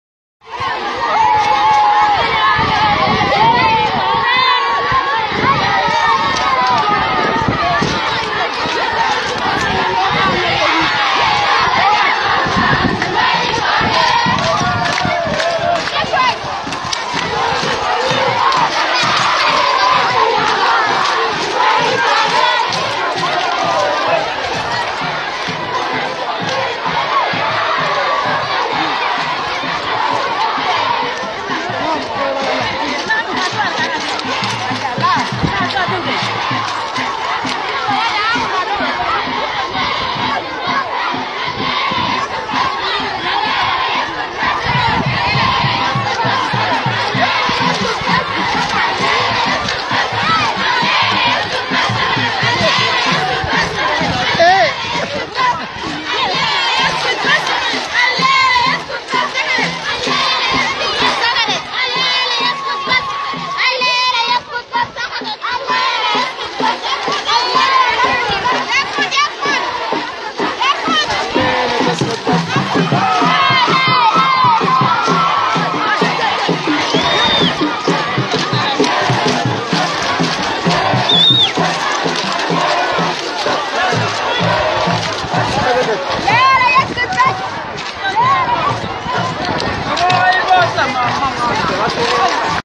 مظاهرات معسكرات النازحين في دارفور في 6 ابريل
مظاهرات 6 ابريل لاسقاط نظام عمرالبشير 6ابريل 2019